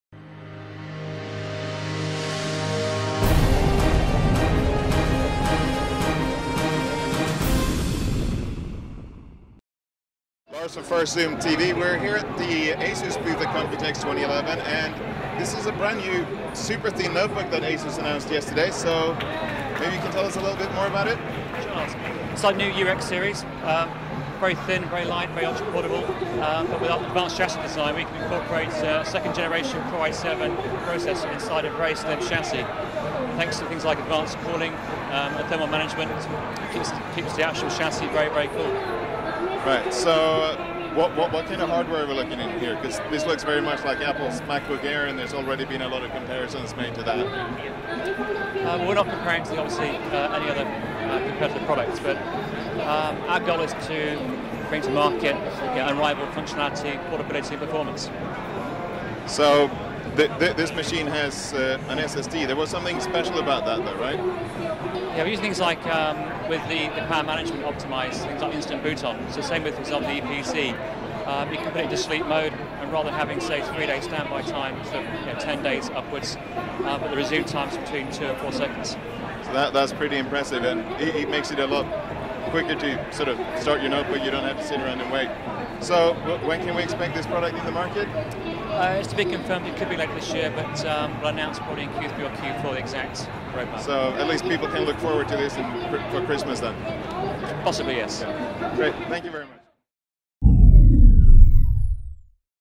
ASUS UX-series notebooks - Computex 2011
UX series notebook with Intel core i7 processors in a super slim chassis with advanced cooling and thermal management keeping the chassis extremely cool. The focus is on unrivalled functionality, reliability and performance. The SSD has instant boot functions where it can boot up between 2-4 seconds. Production lined up for Christmas.